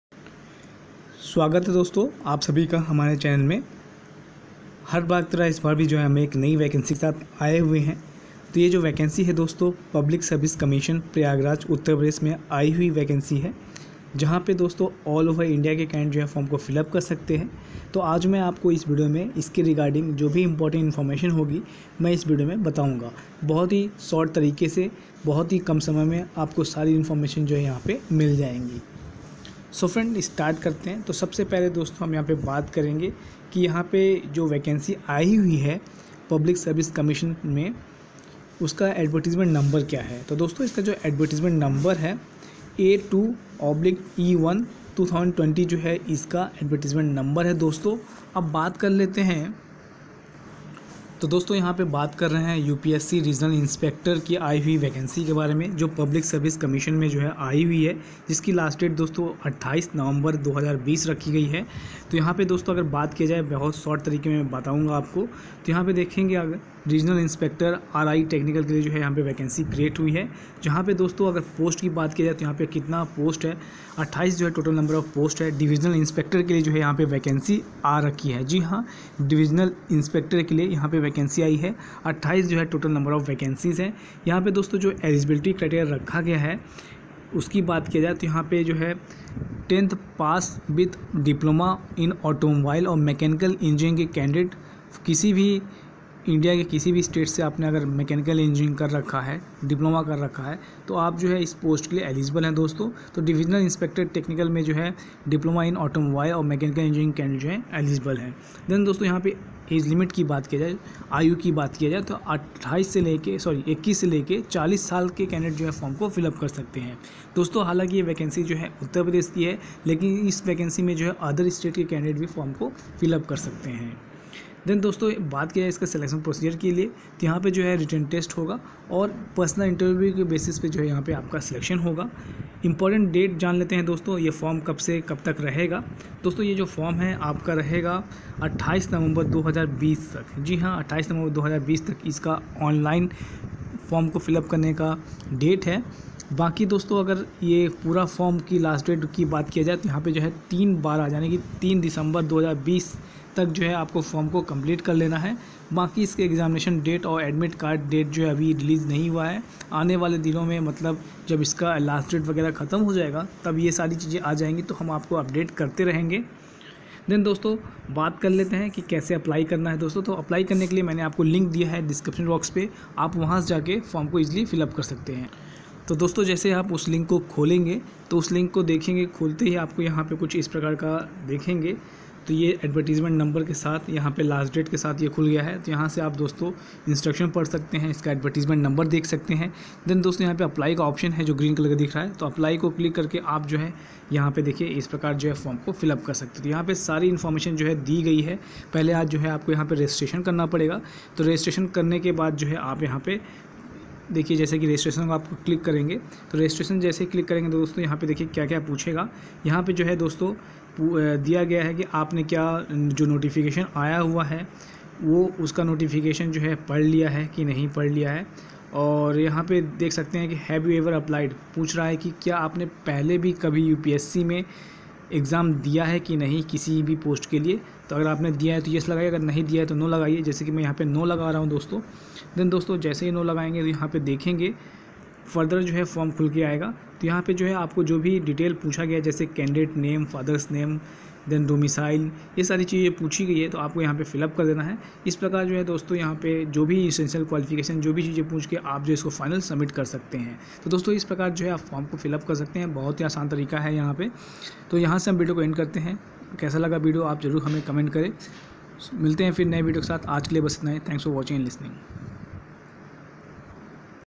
UPPSC Regional Inspector RI Technical Recruitment 2020 - Salary -46000
UPPSC Regional Inspector RI Technical Recruitment 2020

UPPSC RI Technical Recruitment 2020 is inviting application for the posts of 28 Technical Vacancy 2020 from Diploma & Degree pass candidates interested can apply online before 03 December 2020. Please go through this article and follow each tables for full vacancy details, educational qualification, eligibility criteria, pay scale, salary and How to apply UPPSC job opening at official

UPPSC RI Technical Recruitment 2020 Details

Post details No. of Vacancy
Divisional Inspector (Technical) 28